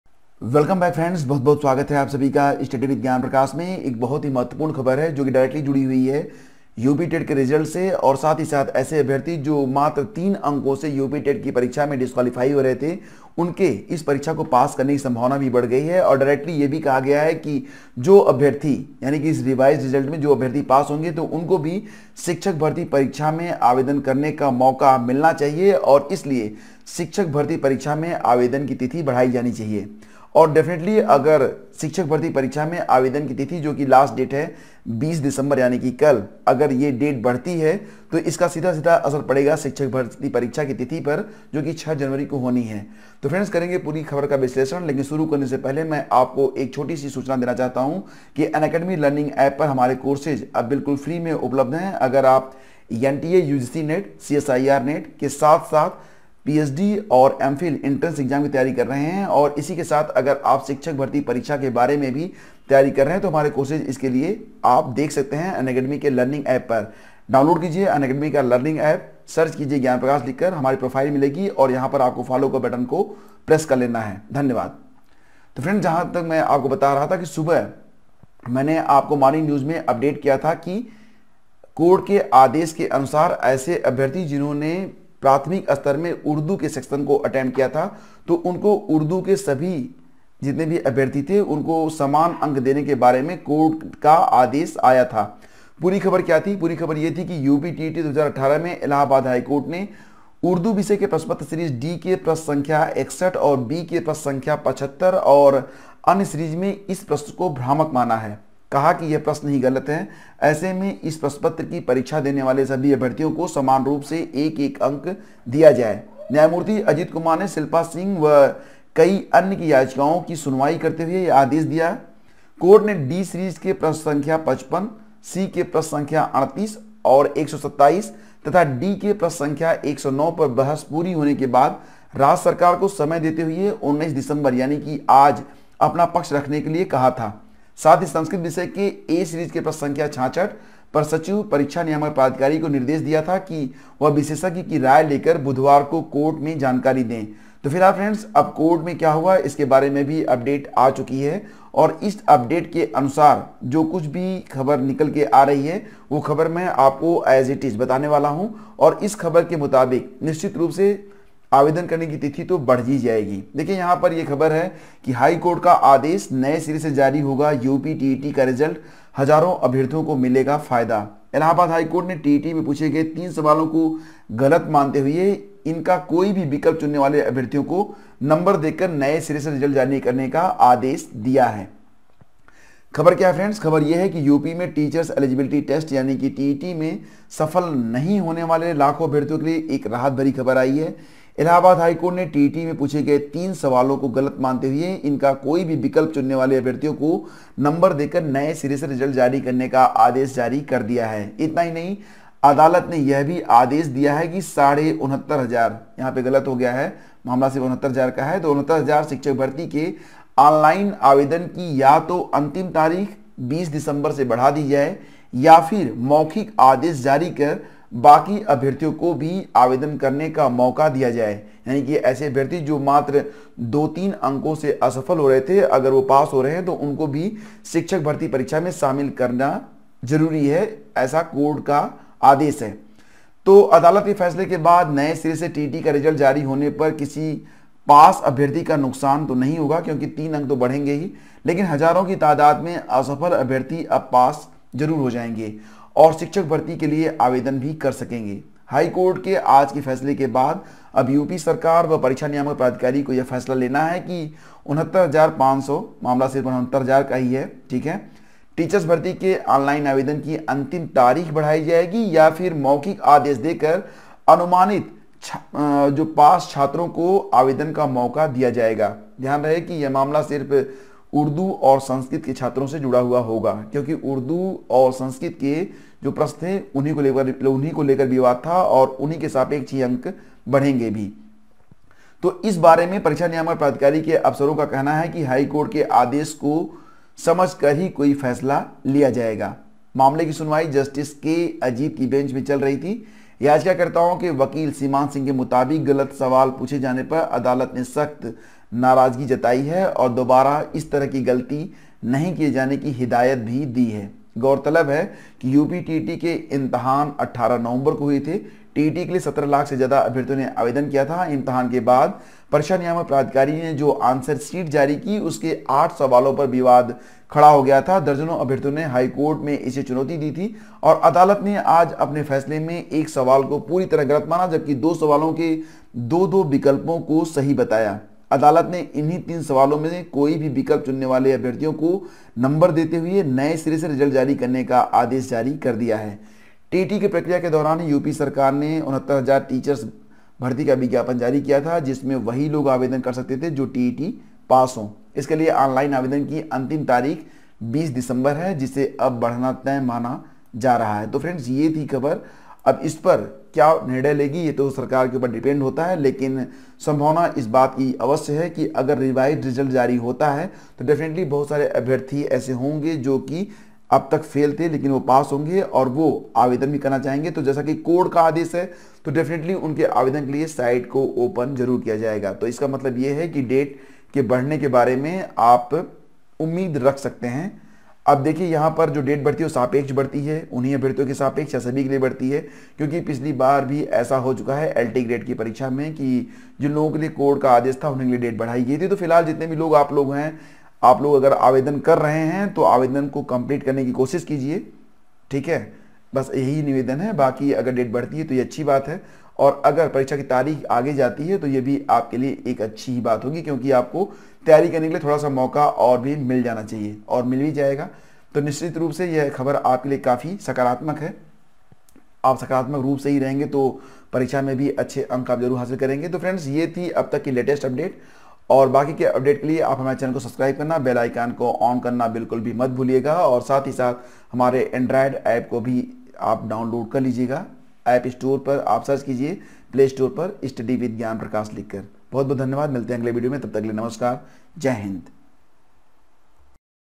UPTET Result दोबारा जारी करनें व शिक्षक भर्ती आवेदन तिथि को बढ़ानें का आदेश
इलाहाबाद हाईकोर्ट नें यूपीटेट के मैटर में तीन प्रश्नों को गलत मानते हुए फिर से रिजल्ट जारी करनें का आदेश जारी किया है और 69000 शिक्षक भर्ती परीक्षा में रिवाइज्ड रिजल्टमें पास होनें वाले अभ्यर्थियों को शामिल करनें के लिए 69000 शिक्षक भर्ती की आवेदन तिथि को बढ़ाने का आदेश जारी किया है।
पूरी खबर का सटीक विश्लेषण
हमें अनअकेडमी पर FOLLOW करें-

हमारे एंड्रायड एप्प को डाउनलोड करें, रहें एकदम अपडेट
Thank you so much to see the description
Study with Gyan Prakash